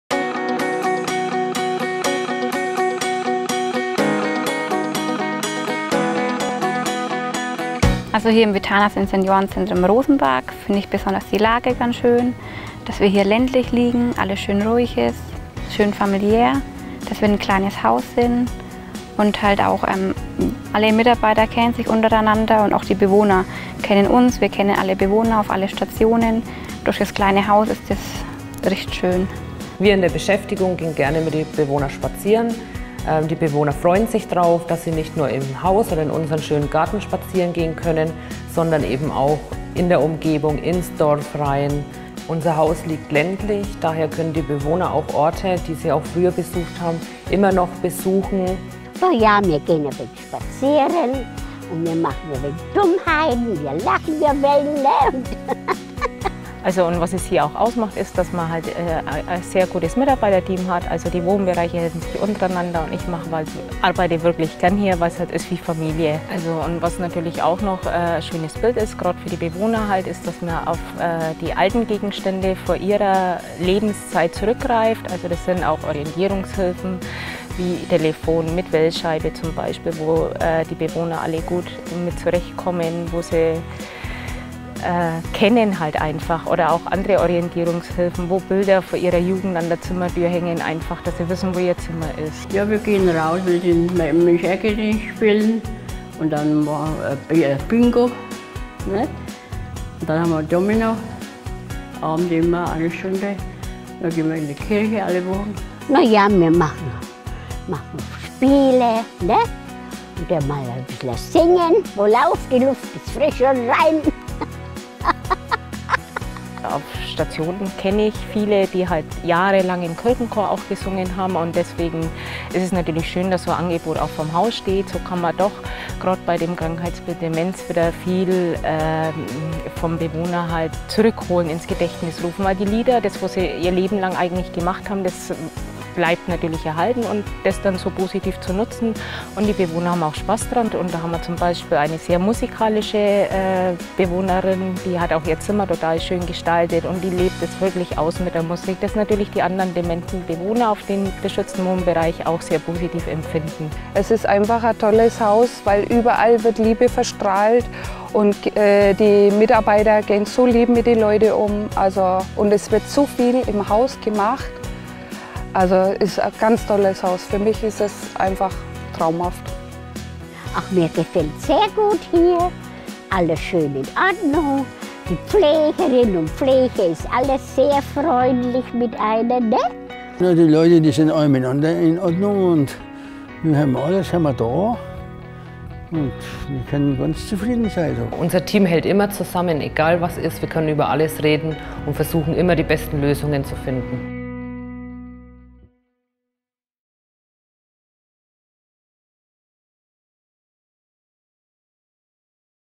Vitanas Senioren Centrum Rosenpark
Herzlich willkommen!
Das Vitanas Senioren Centrum Rosenpark verfügt über insgesamt 79 Plätze auf zwei Etagen in gemütlichen Einzel- und Doppelzimmern. Alle Zimmer sind barrierefrei jeweils mit eigenem Duschbad ausgestattet. Zur Grundausstattung gehören selbstverständlich Telefon- und TV-Anschluss sowie eine Notrufanlage.

Elektrisch höhenverstellbare Betten und den passenden Nachttisch stellen wir ebenso wie einen Kleiderschrank zur Verfügung. Gerne können Sie auch Ihr eigenes, vertrautes Mobiliar mitbringen. Wir helfen Ihnen bei der Einrichtung – schließlich möchten wir, dass Sie sich in Ihrem neuen Zuhause wohlfühlen.

Unsere ganz besonderen Angebote:
- Spezieller Wohnbereich für Menschen mit Demenz
- Wunderschöner Garten mit Ruhezonen, welche zum Verweilen und Entspannen einladen
- Einzelne Zimmer im Erdgeschoss mit eigener Terrasse
- Viele bunte und abwechslungsreiche Veranstaltungen für Jung und Alt